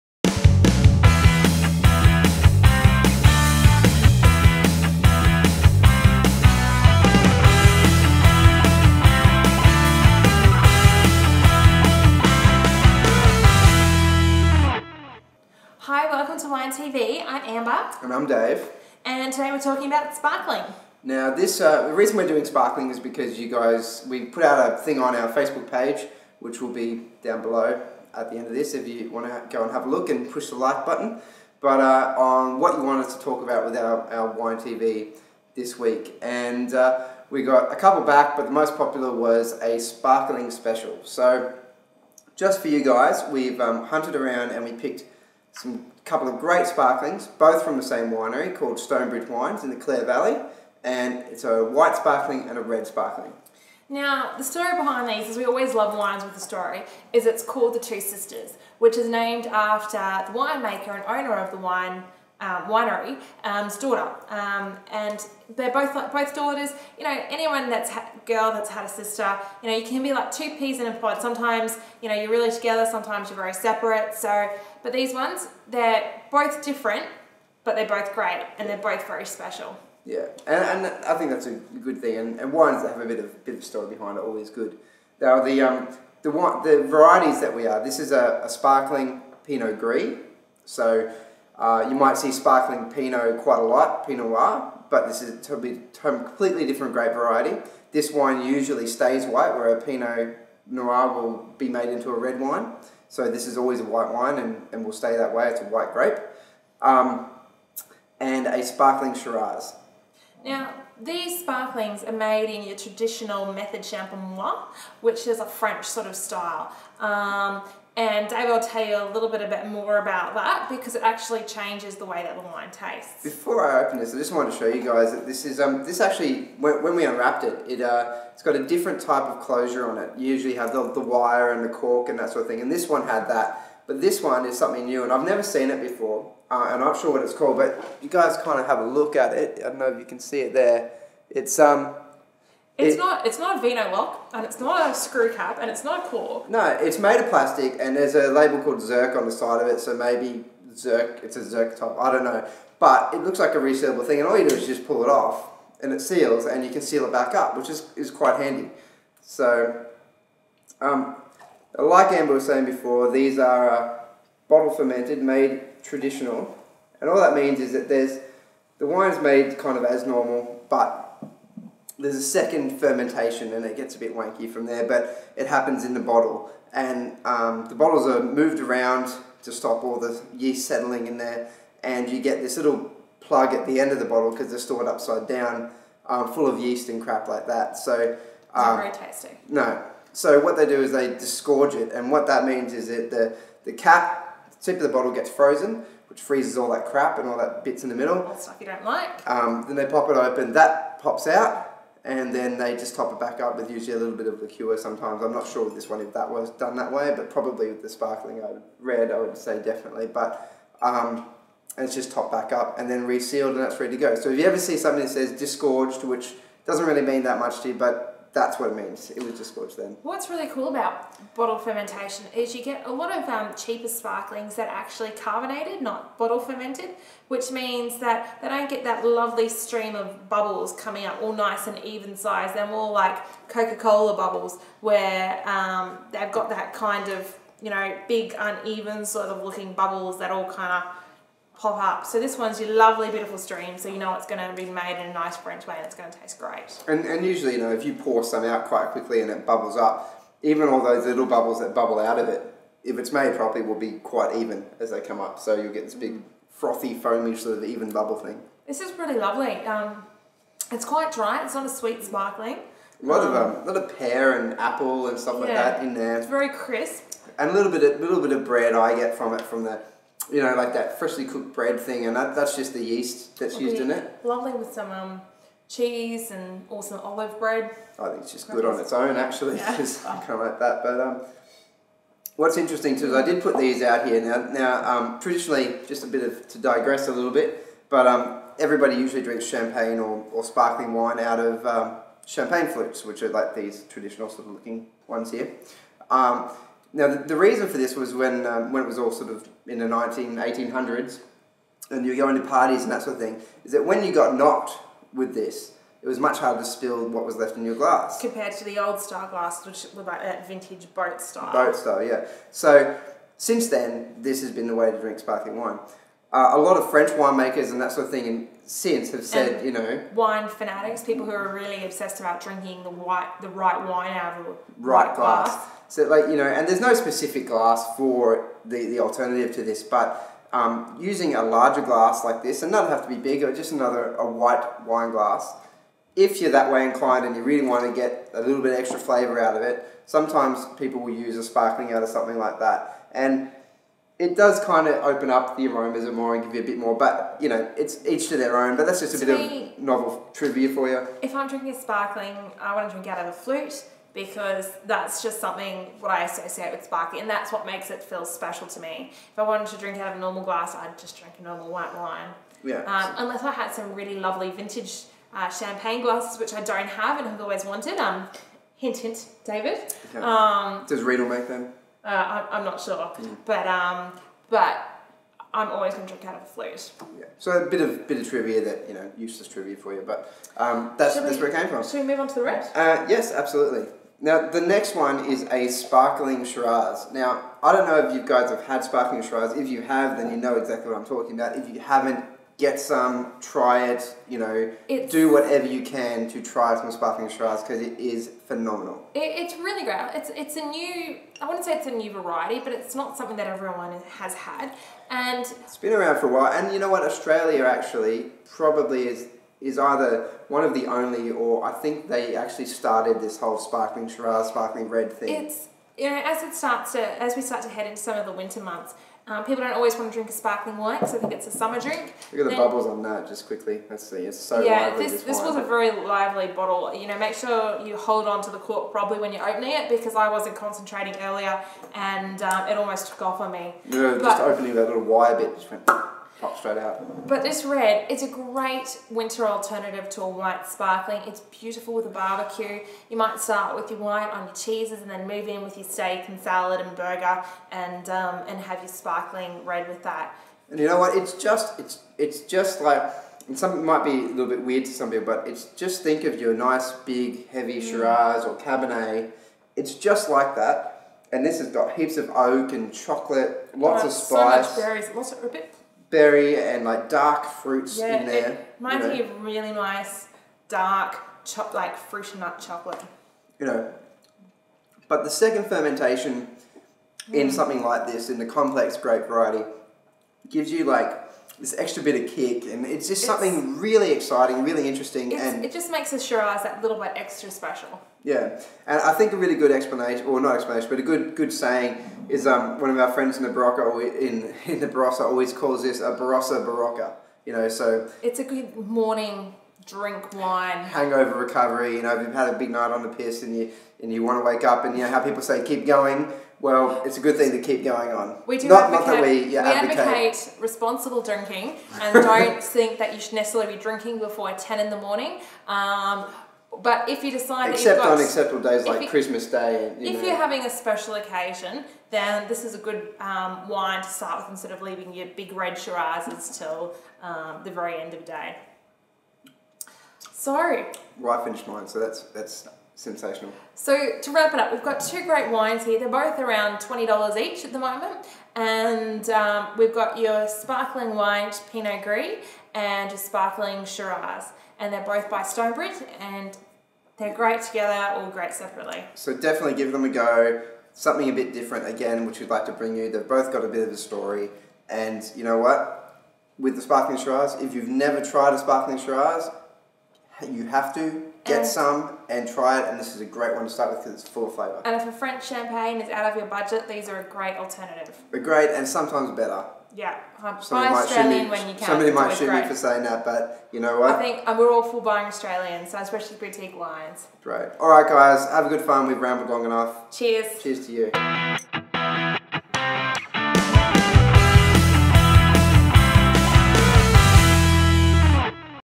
Wine TV Episode 7-Sparkling Special
Wine Buzz taste some epic sparkling wines from Stone Bridge Wines in the Clare Valley. Sparkling Pinot Gris and a Sparkling Shiraz. These wines kick some serious butt. Take a look pop some sparkling and make some comments. Hope you enjoy